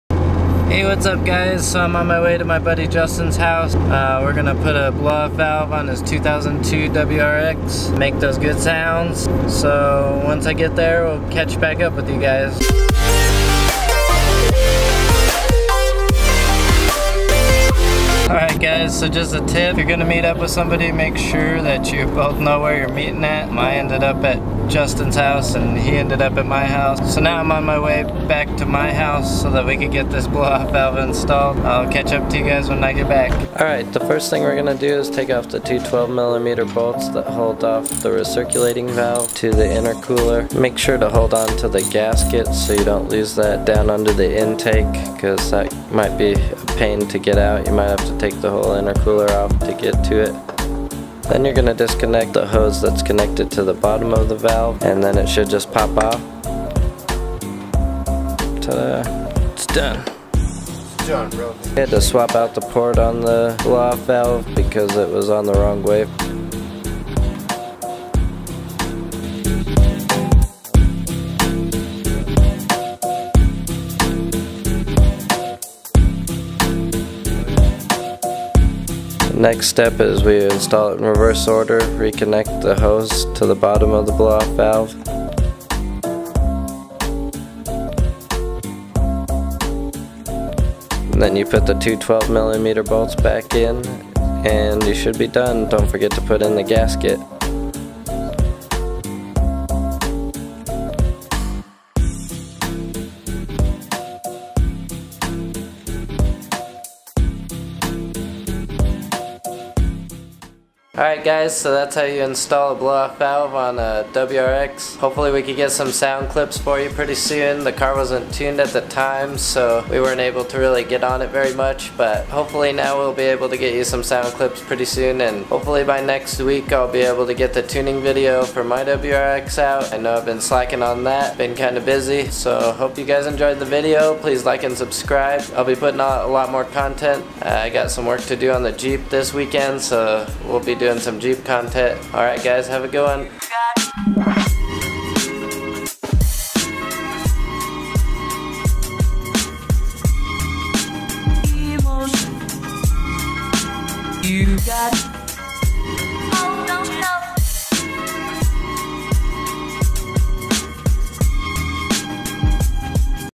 How to: Installing a Blow Off Valve on a 2002 WRX
Hey Guys,

In today video we install a Turbosmart 50/50 blow off valve on my friend Justin's 2002 Subaru WRX bugeye. Super easy install and if you get a 50/50 blow off valve you will not need a tune. This blow off valve install is the same for most if not all years of WRX and STI. Hope you guys enjoy.